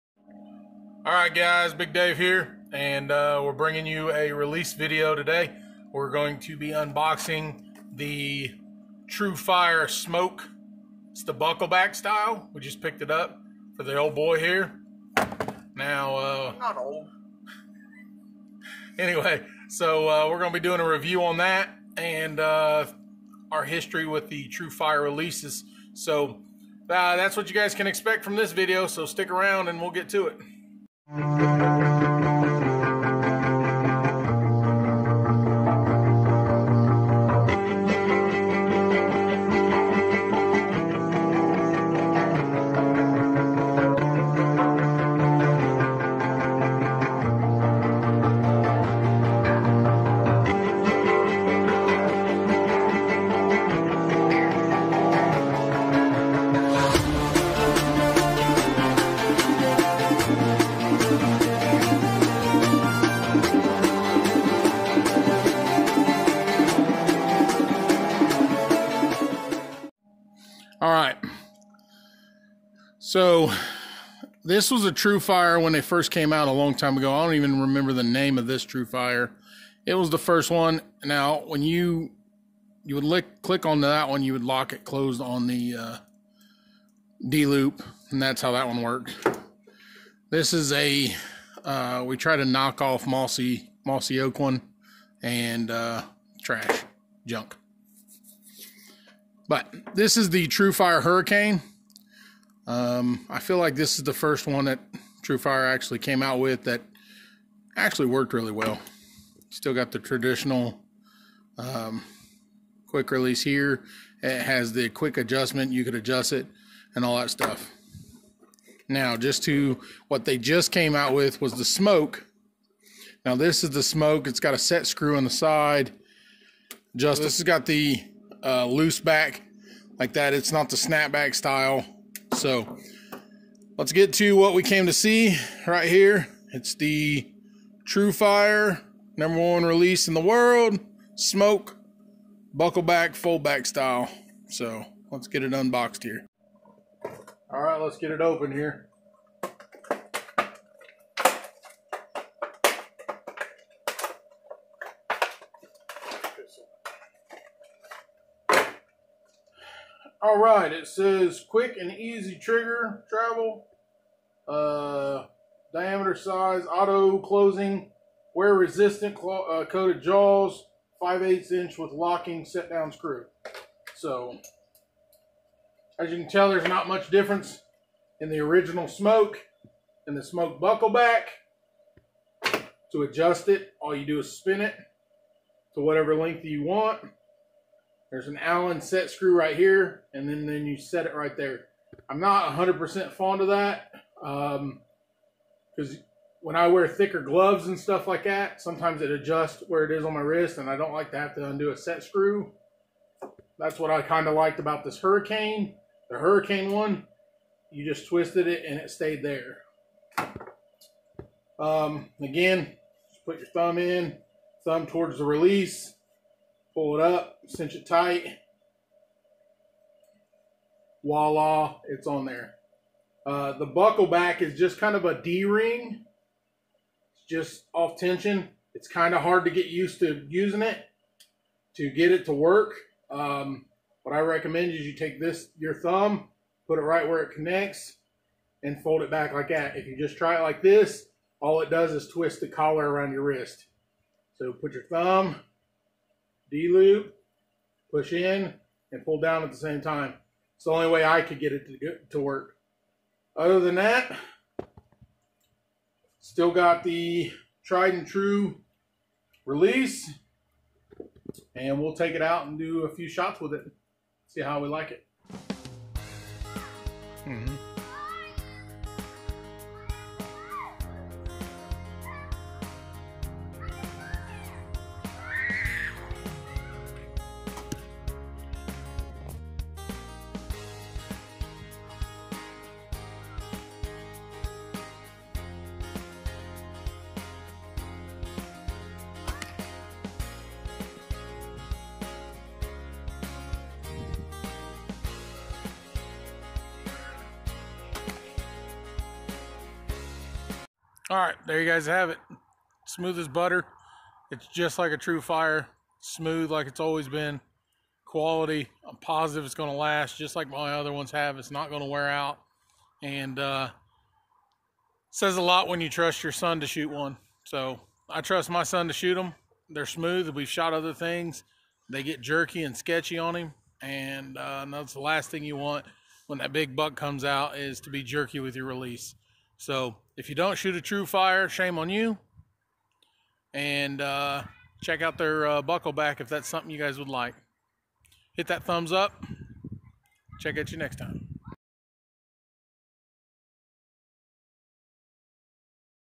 TruFire Smoke Buckle Back Release
TruFire Smoke Buckle Back Release, we go over what i don and dont like about this release. Like normal the trufire band has came to the table with a solid release. Its smooth like normal. But the Buckle back is a little difficult to use.